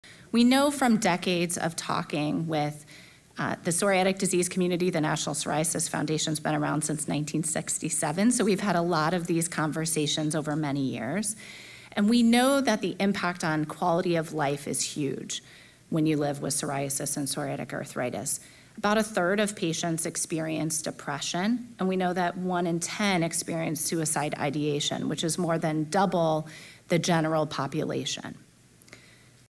Novel Therapies for Plaque Psoriasis
Explore the impact of plaque psoriasis, the value of treatment options from the patient perspective, clinical evidence for novel therapies, and managed care considerations to optimize access to appropriate treatment. Earn 1.25 hours of continuing pharmacy education at no cost by completing the full program through AMCP Learn

Faculty Speaker:
Leah M. Howard, JD
President & CEO
National Psoriasis Foundation

The Academy of Managed Care Pharmacy (AMCP) is accredited by the Accreditation Council for Pharmacy Education as a provider of continuing pharmacy education.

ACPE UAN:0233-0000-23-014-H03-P
Activity Type: Knowledge-Based
Contact Hours: 1.25